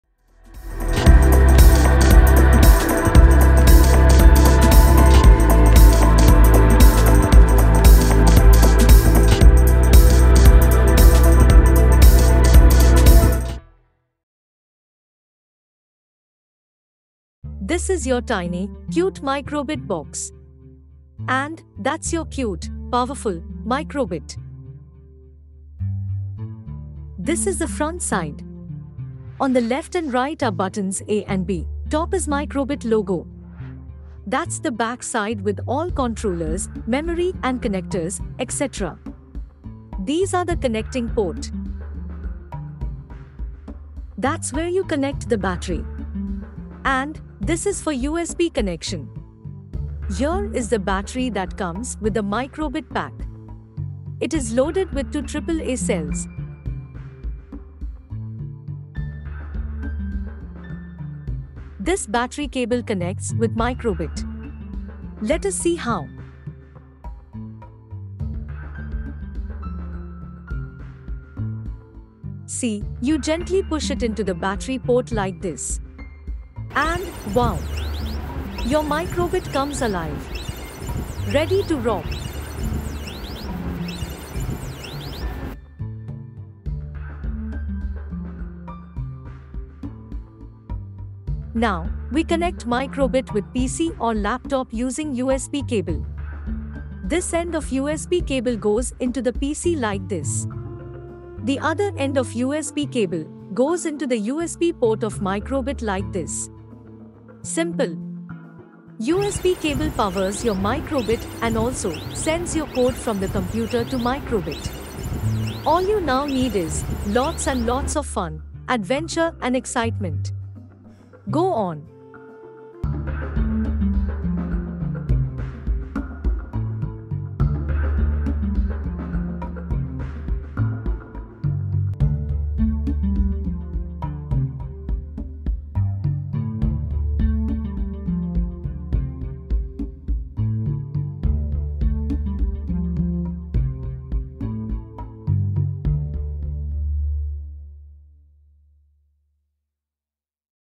Connecting and Powering Up Micro:bit
Quickly learn how to power up Micro:bit and connect with PC.